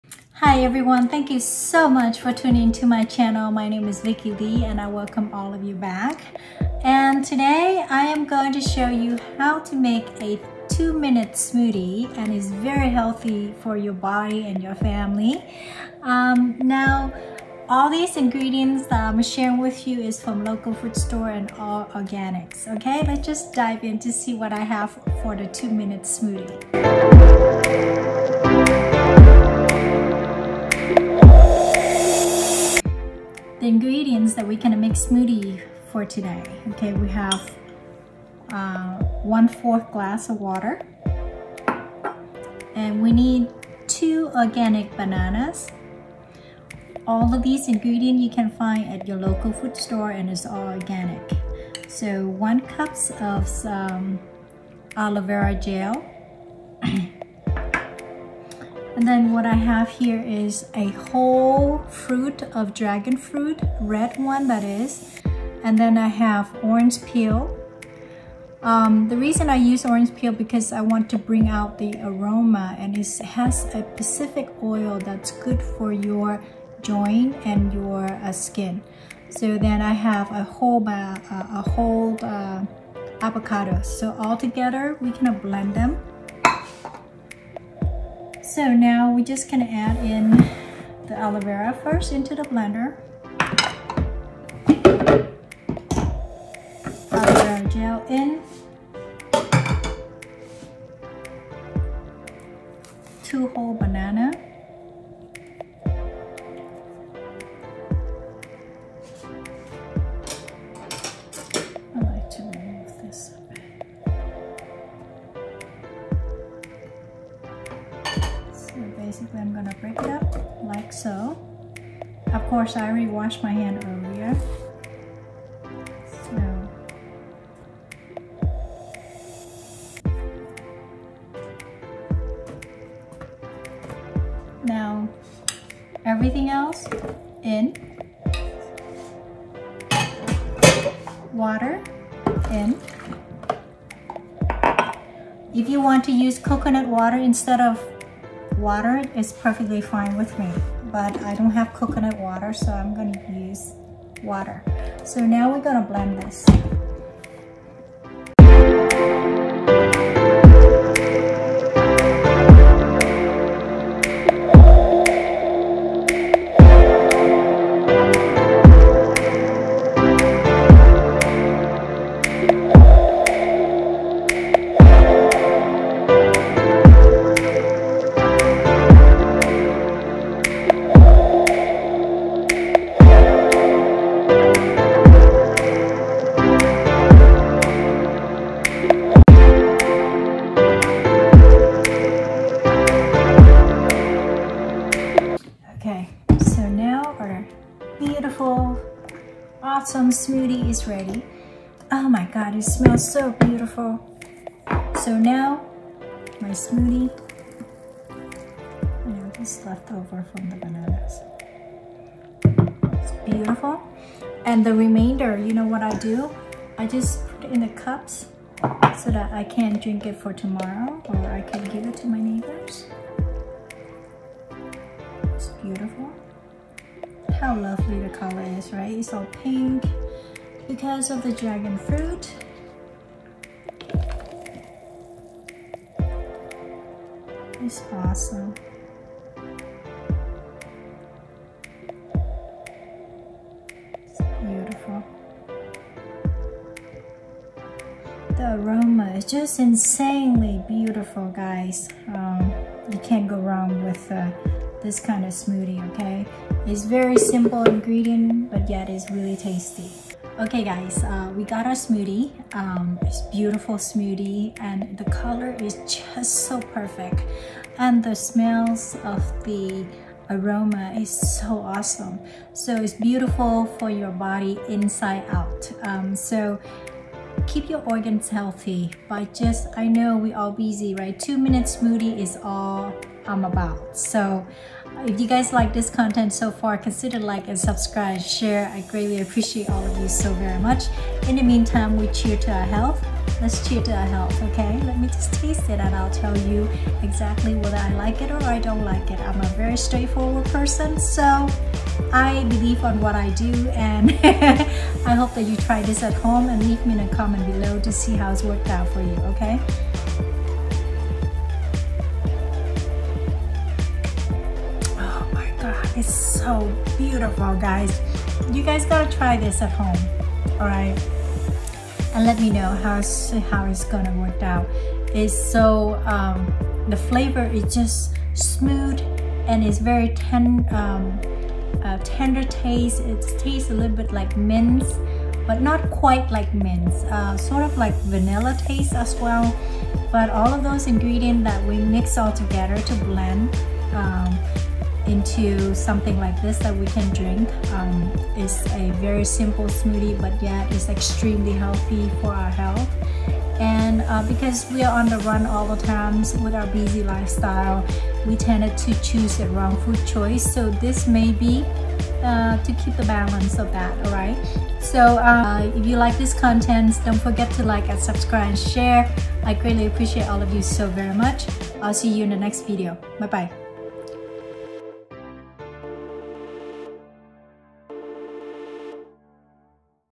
2 MINS DRAGON FRUITS WITH ORANGE PEELS SMOOTHIE#delicious
A delicious smoothie pack full of natural Vitamins to nourish our body for excellent health.
HOW TO MAKE A 2 MINS DRAGON FRUITS W/ORANGE PEEL SMOOTHIE
Ingredients:  All Organic Ingredients are preferred
1 Whole Dragon Fruits Red or White
1 Whole Avocado
1 Cup of Aloe Vera Gel
2 Whole Organic bananas
¼ Cup of Water or Coconut water
1 slide of Orange peel

Take care, and I hope this helps!
Sending you all much Love

Vicky Lee

Remember to keep comments respectful and to follow our Community Guidelines.